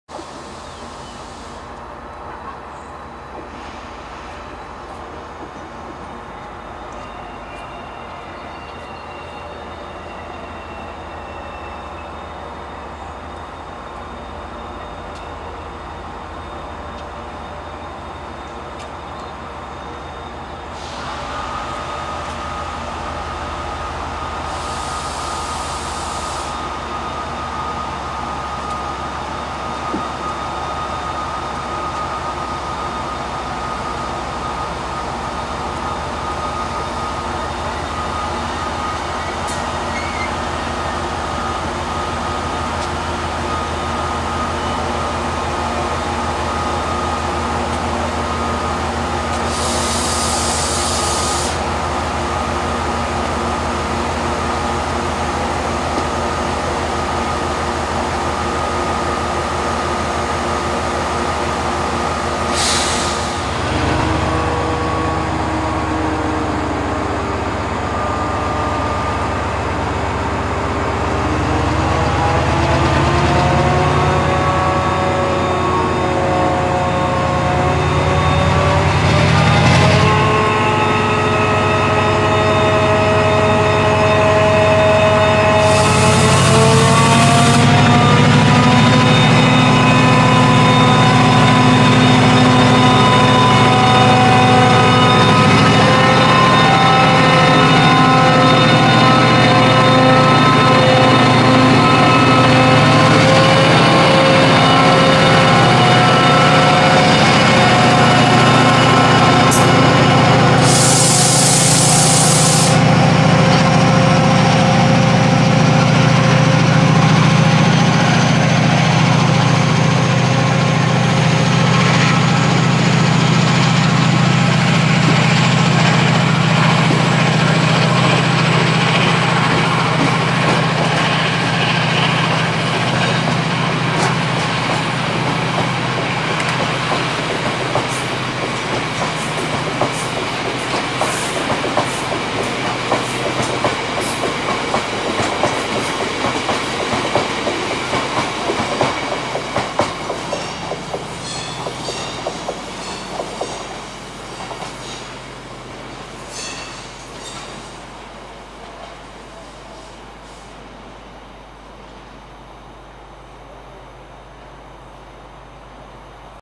68016 restarts ballast train, Stafford 16/12/15 (bit of noise!)
6U77 Mountsorrell - Crewe. Pleased there were no problems with this shot, unlike some of my others!!